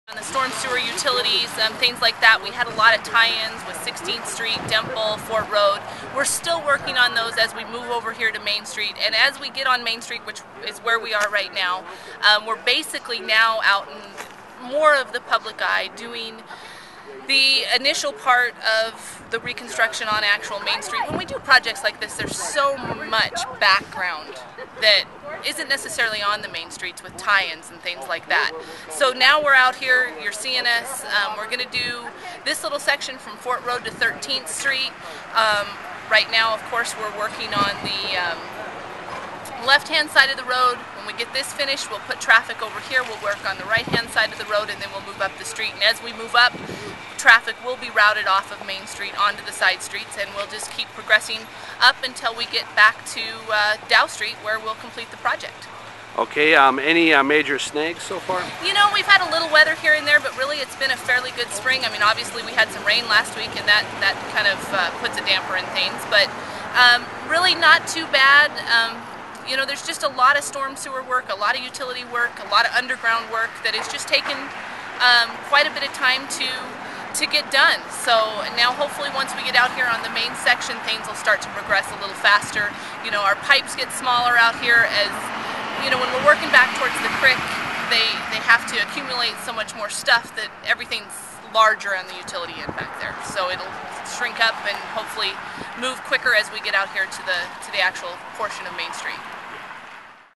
Ronda Holwell - WYDOT North Main reconstruction
WYDOT Public Involvement Specialist Ronda Holwell discussing the North Main street reconstruction project.